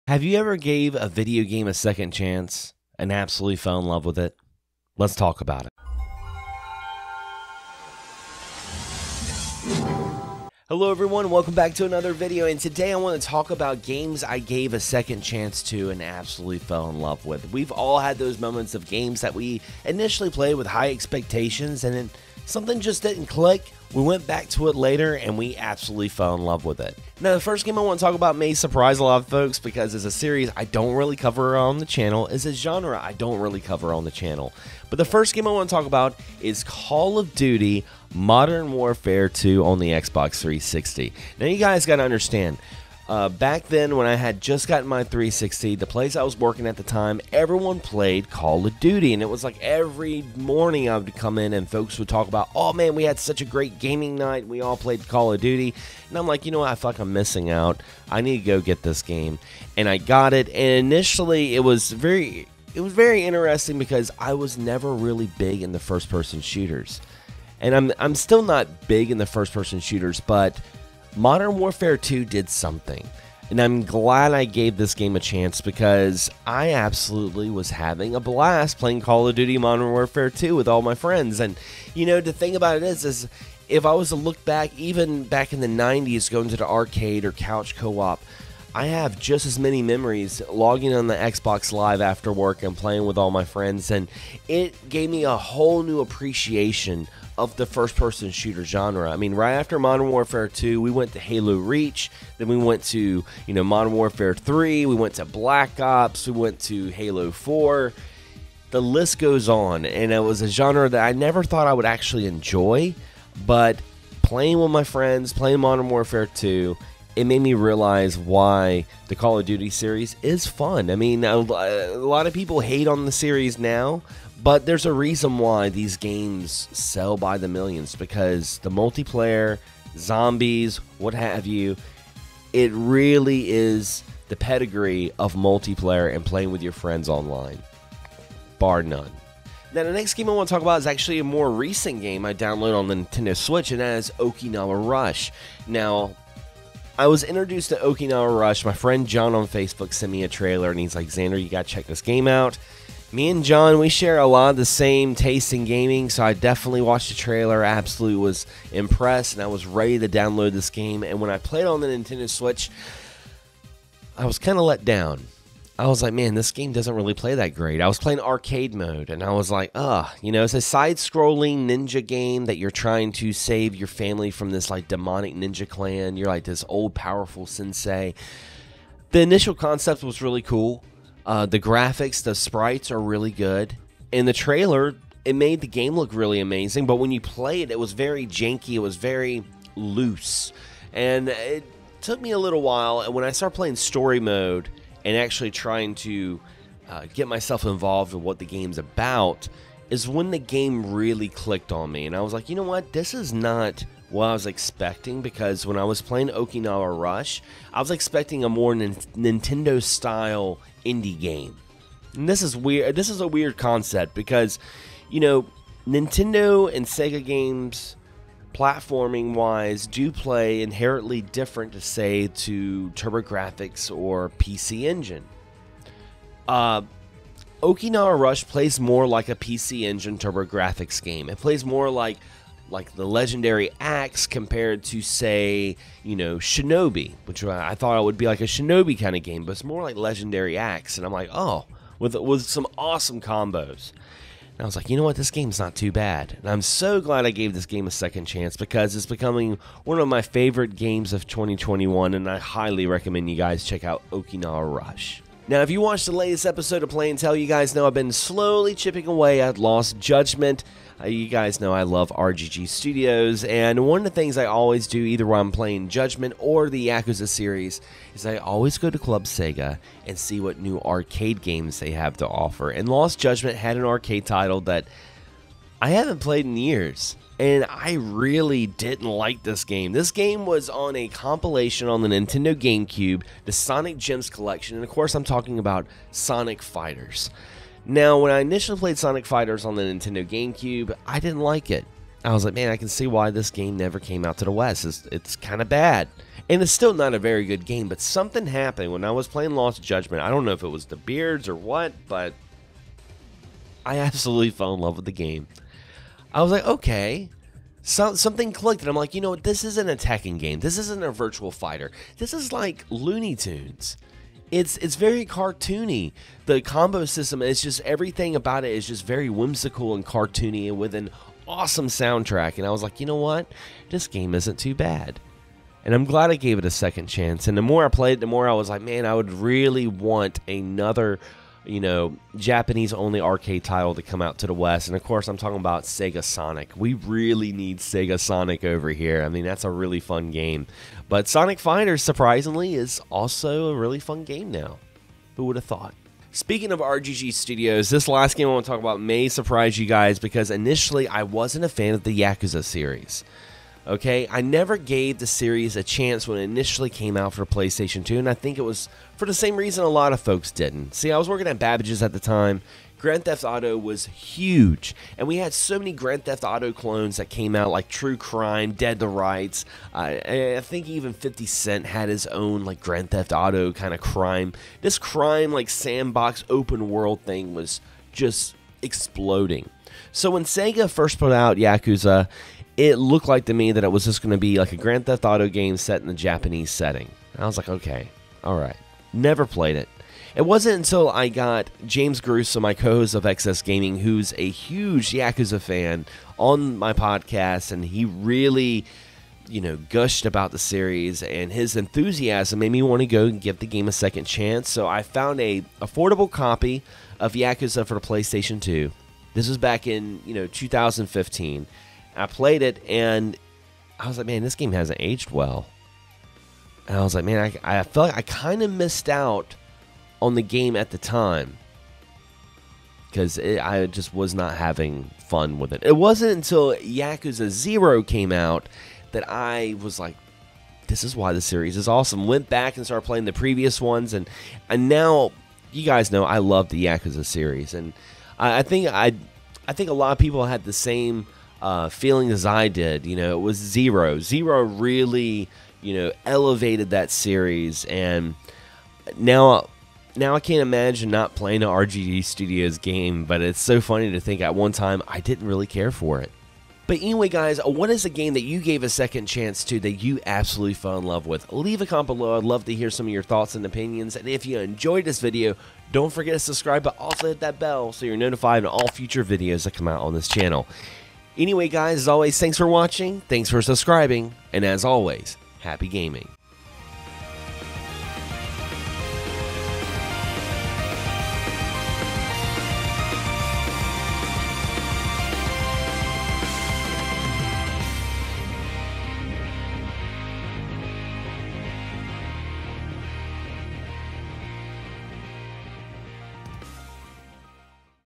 Giving a Video Game a Second Chance
Have you ever played a game and it just didn't "Click"? Only to play it later and love it? Some of my Titles might surprise you! Let's talk about it!
also TikTok @xanderskullion